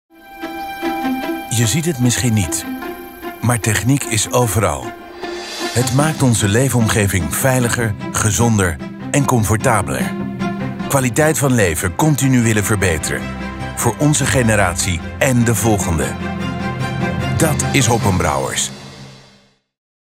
Techniek is overal.. Het maakt je leefomgeving veiliger, gezonder en comfortabeler.
Je ziet het misschien niet, maar techniek is overal.
Het maakt onze leefomgeving veiliger, gezonder en comfortabeler.
Kwaliteit van leven continue willen verbeteren, voor onze generatie én de volgende...
Dát is Hoppenbrouwers. Dát is waar techiek leeft.